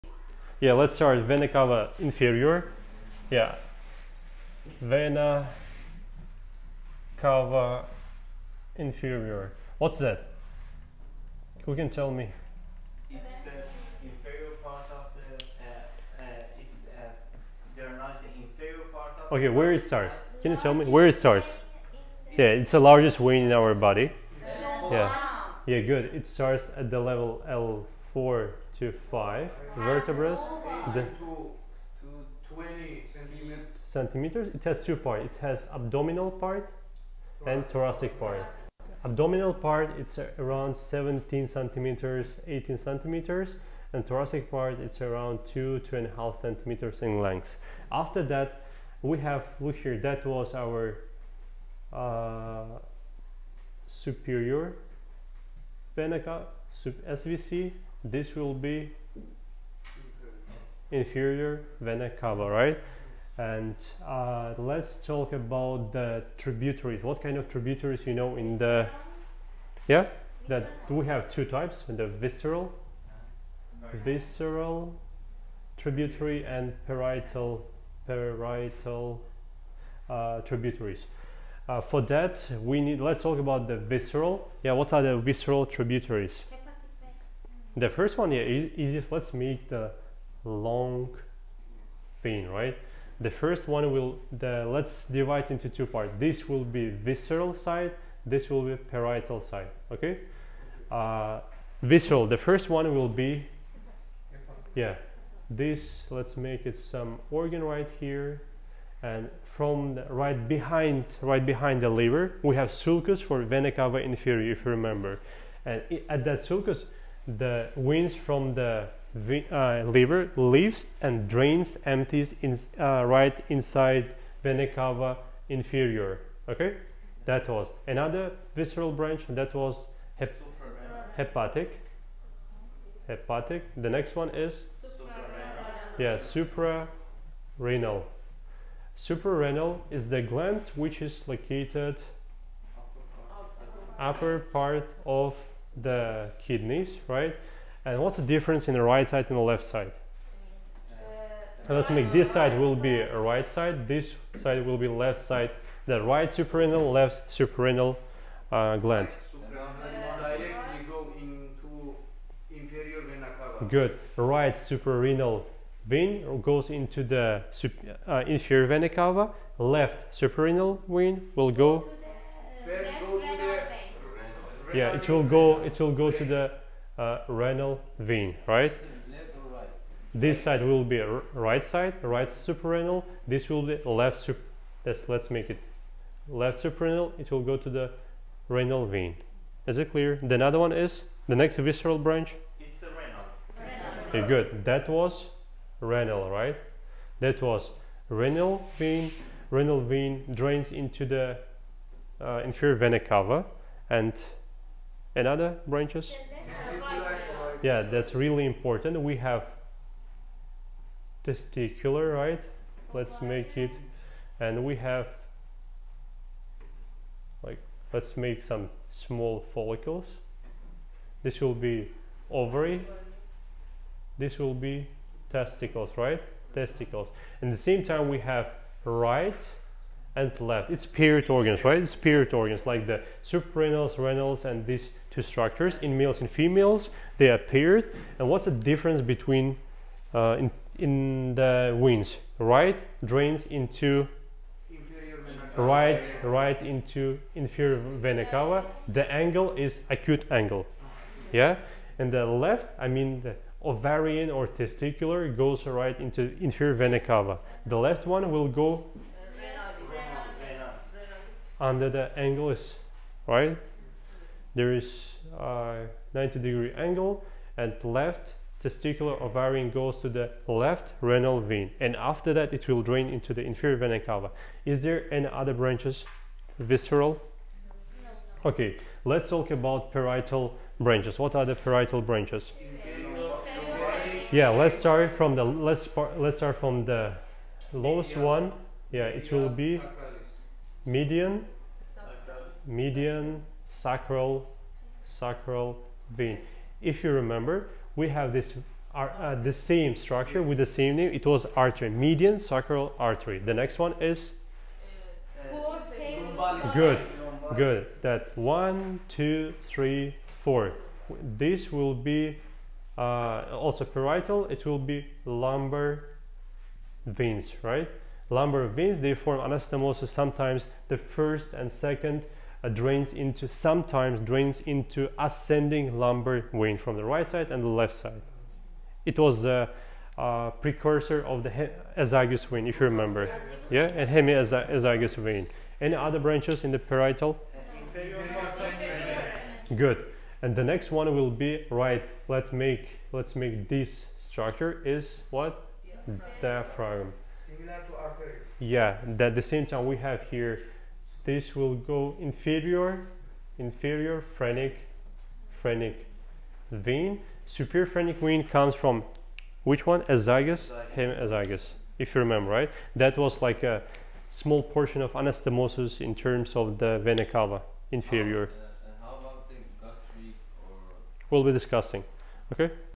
INFERIOR VENA CAVA: Anatomy and Function
Welcome to my latest video about the human body! In this video, I'll be talking about an important blood vessel that runs through the abdomen and carries deoxygenated blood from the lower part of the body back to the heart. That vessel is called the Vena Cava Inferior.

First, I'll provide an overview of the circulatory system and why blood flow is so crucial to our bodies. Then, I'll delve into the anatomy of the Vena Cava Inferior, discussing its length, diameter, and branching patterns. You'll learn where it originates and how it eventually connects with the heart.